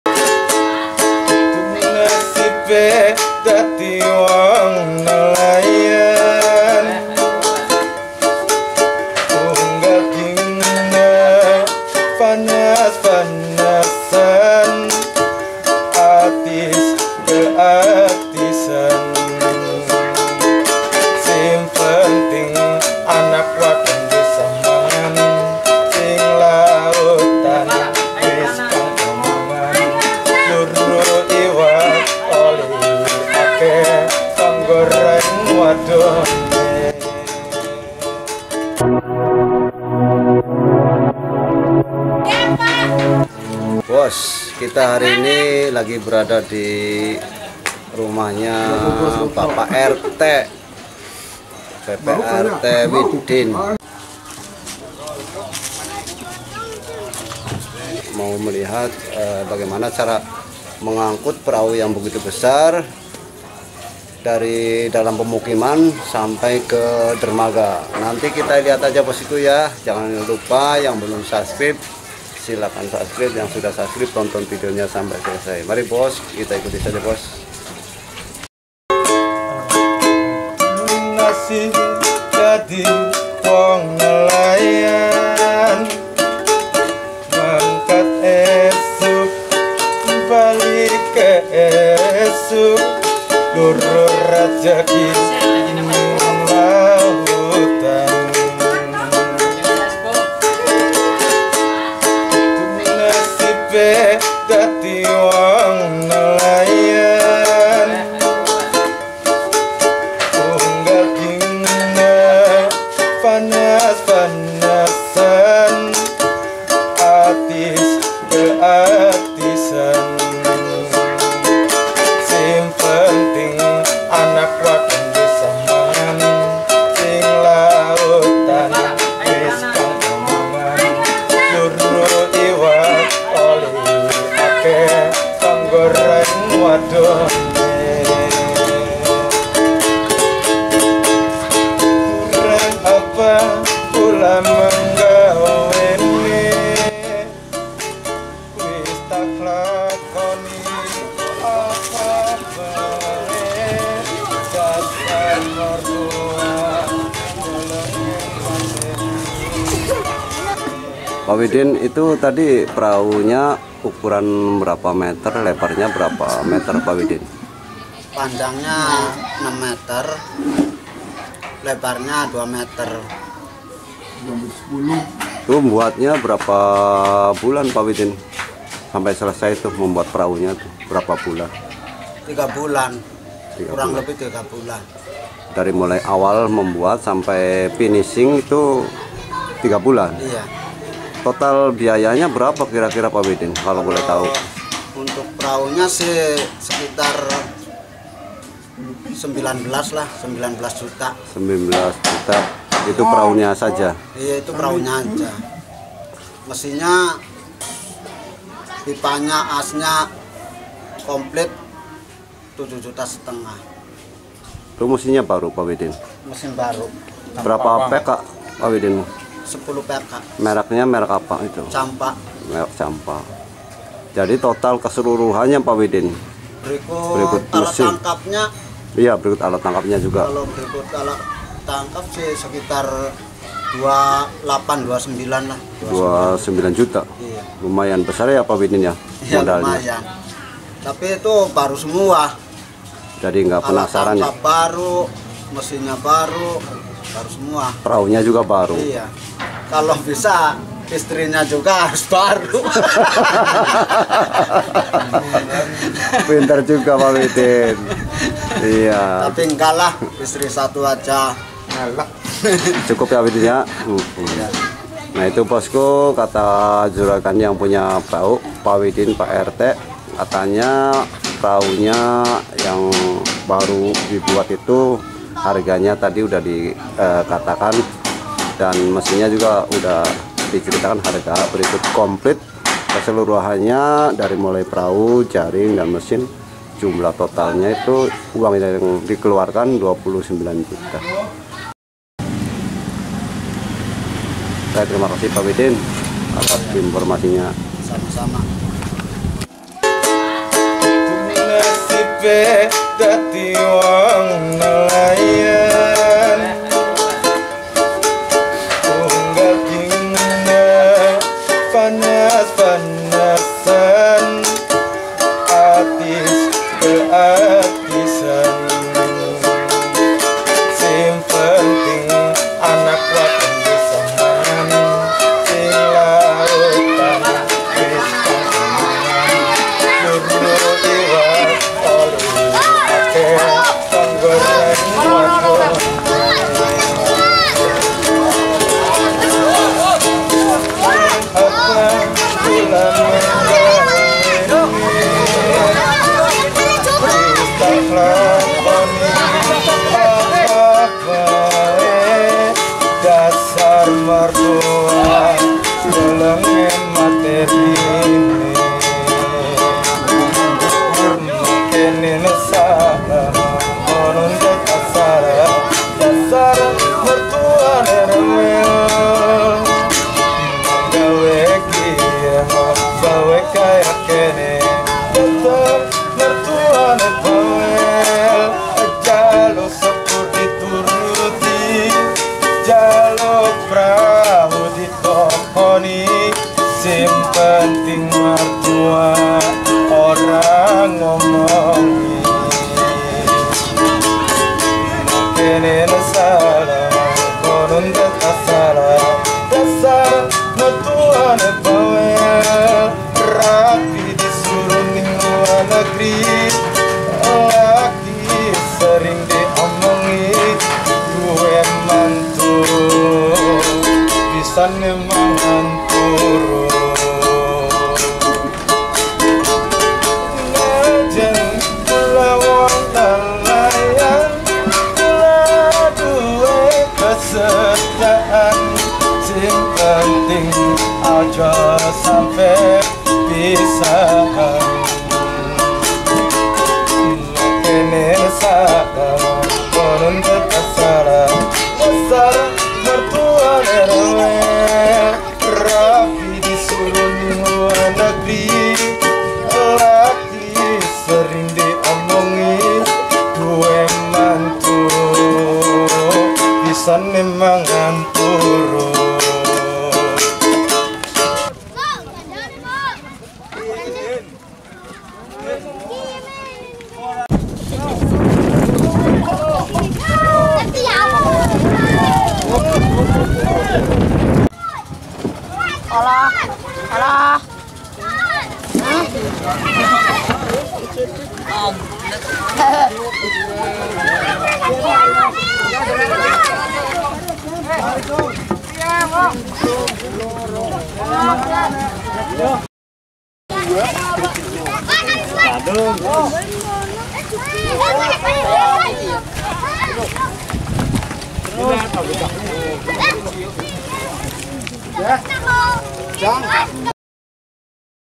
Perahu Besar Keluar Dari Dalam Pemukiman.
Nelayan Warga desa Ambulu ,Kec  losari kab cirebon,
mengeluarkan prahu dari dalam pemukiman Menuju dermaga.